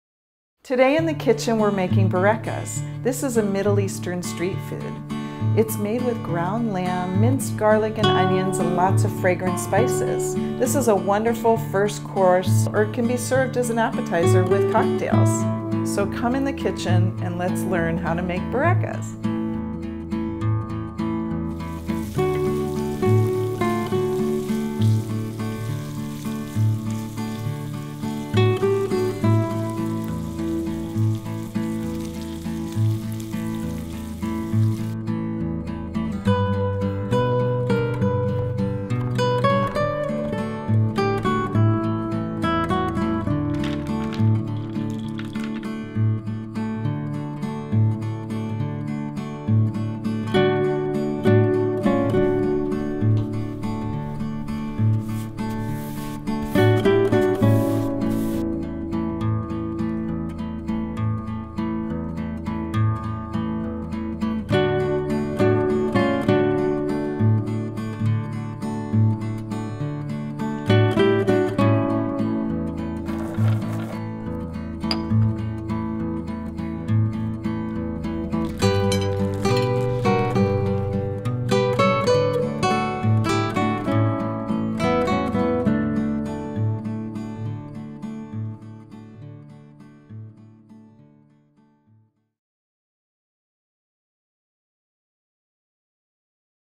Chef Robin - Making Borekas
Come learn how to make Borekas, these handmade meat-filled phyllo rolls served with yogurt, a popular Middle Eastern street food. This lamb-filled variation has been “reinvented” and lightened up using phyllo pastry, sweetened with fragrant spices and baked in the oven rather than fried.
Follow on your favorite social platform